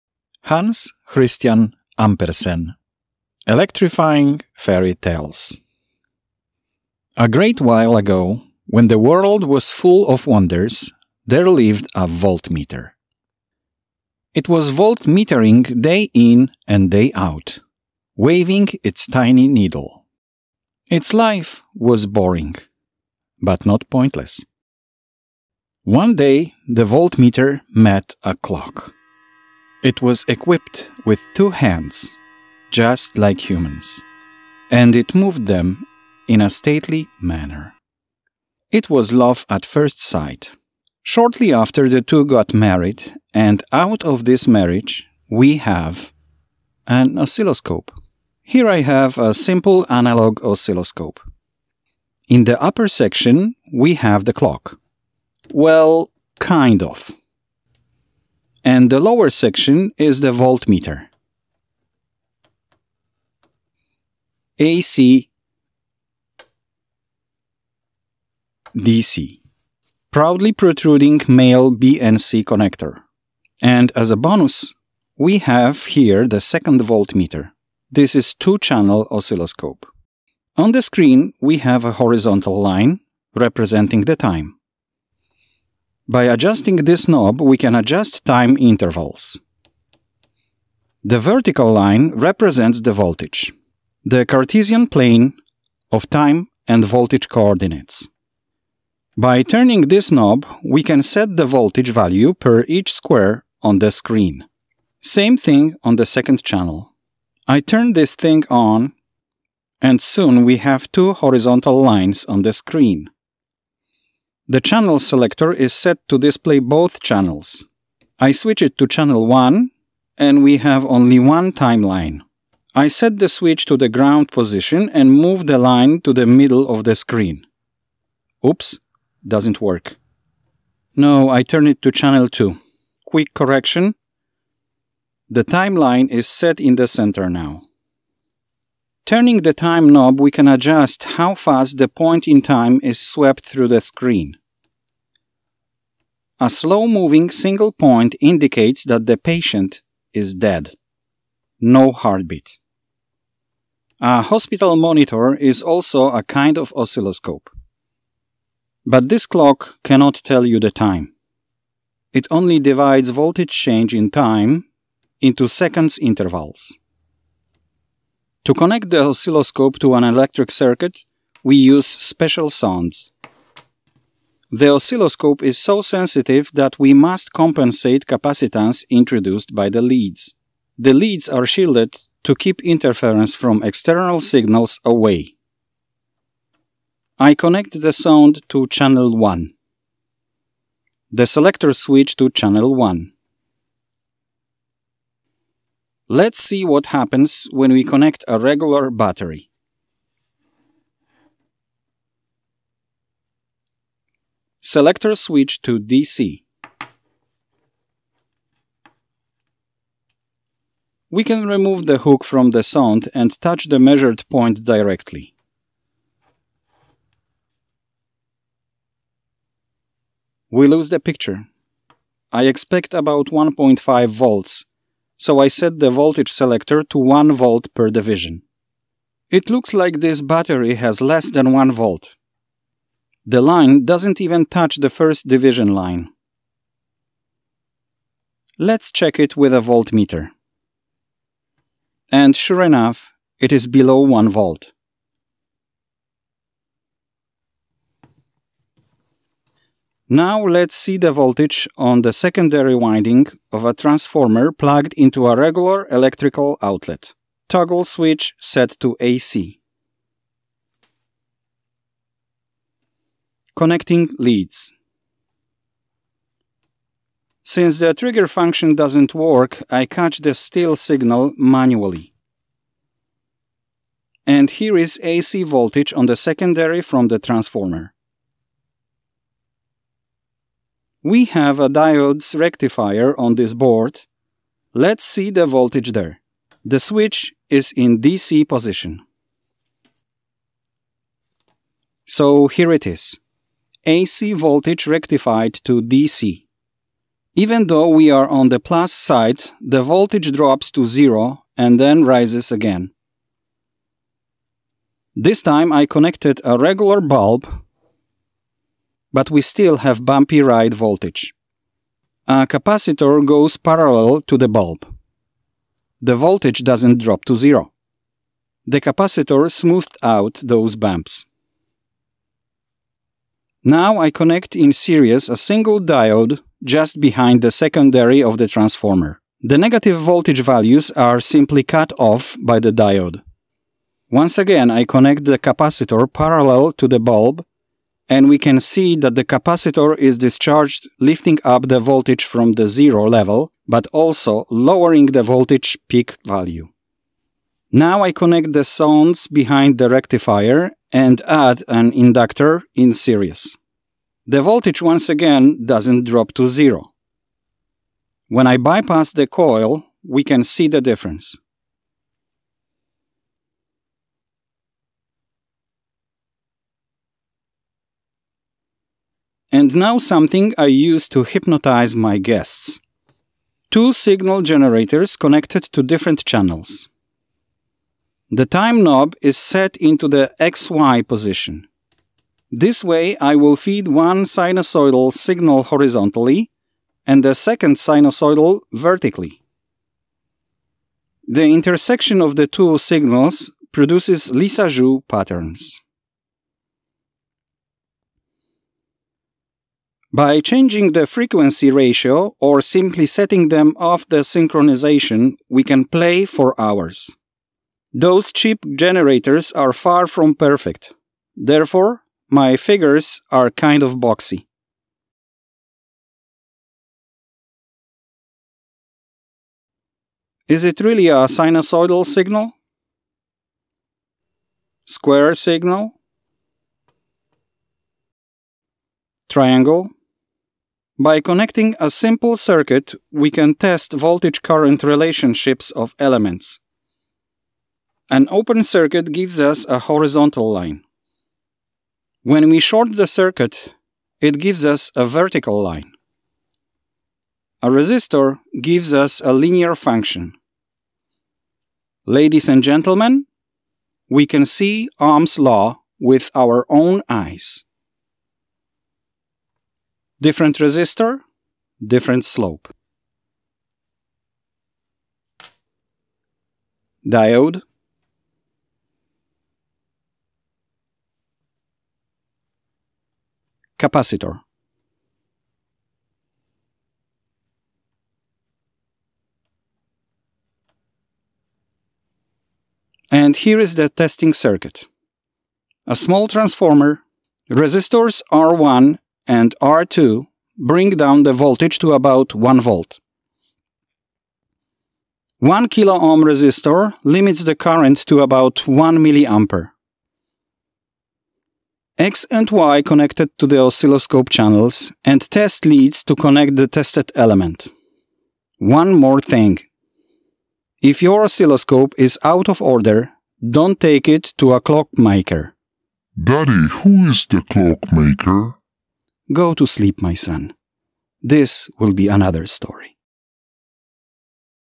Oscilloscope Story (The Real One)
Analog oscilloscope. Lissajous Figures. Elements testing with an octopus.